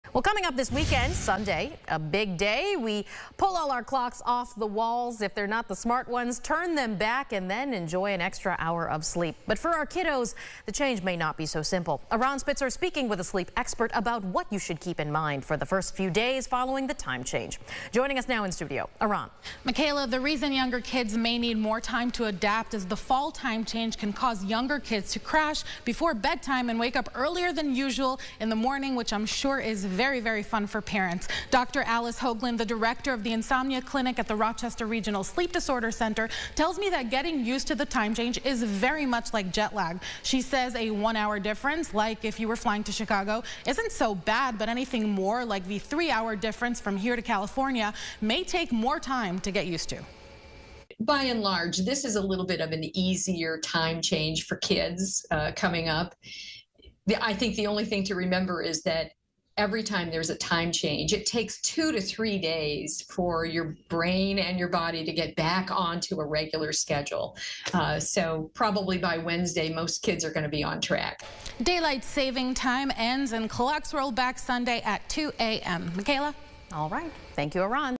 Clocks fall back on Sunday — your kids might wake you up early
Sunday is the big day — we change our clocks, fall back, and enjoy that extra hour of sleep. But for kids, that change may not be as simple.